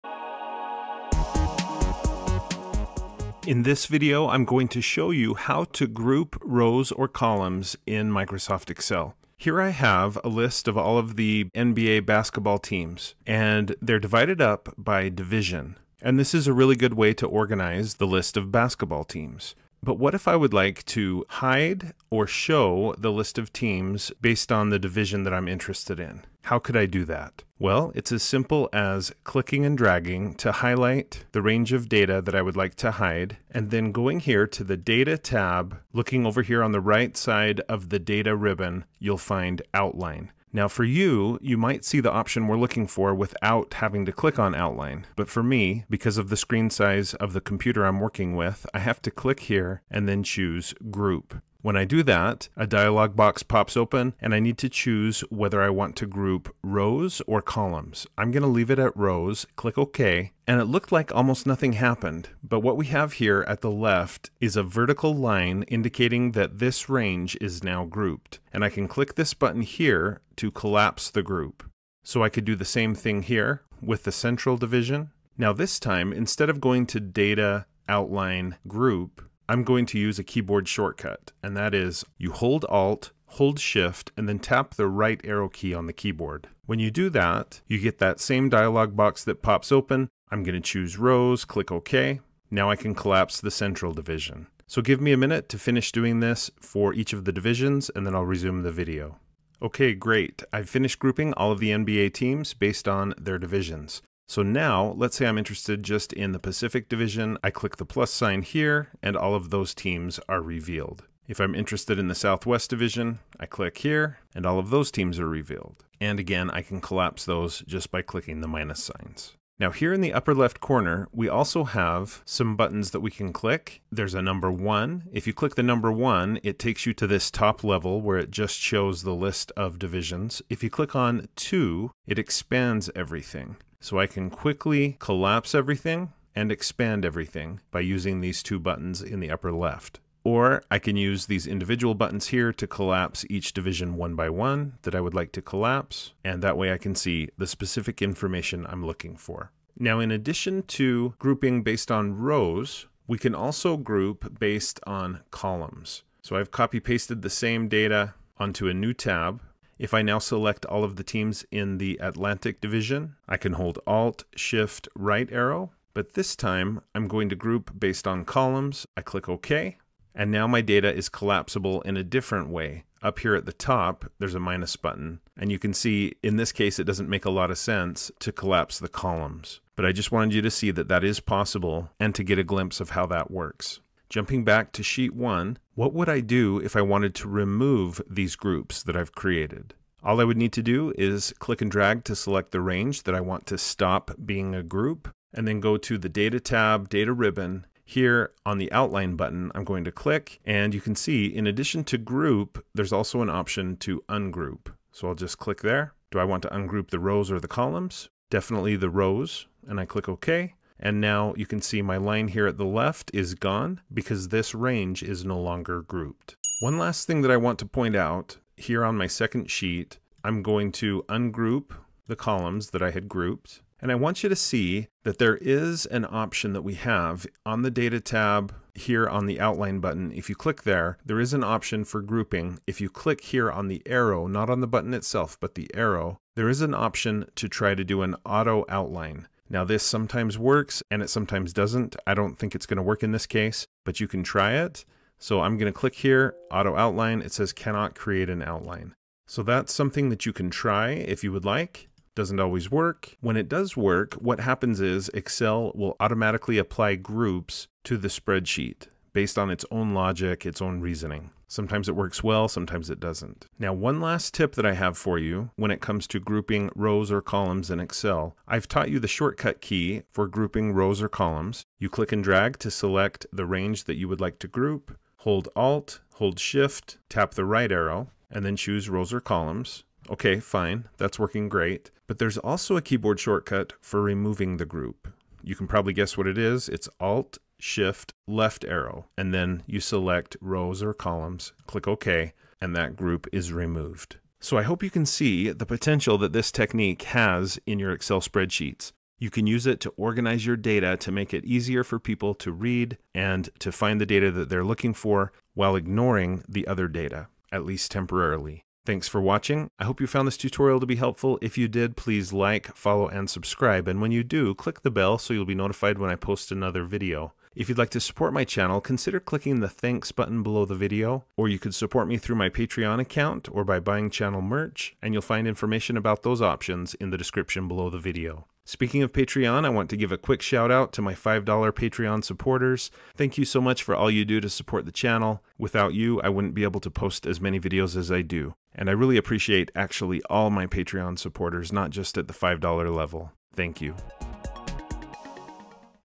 Grouping Rows and Columns in Excel
Learn how to organize your Excel data better by grouping rows and columns. This organizing technique can make it much easier to focus on the specific data in Excel that's relevant to the viewer at a particular time. It's not a hard skill to learn, but there are several key points that you need to understand in order to group rows and columns effectively. Here's a link to the practice file for this video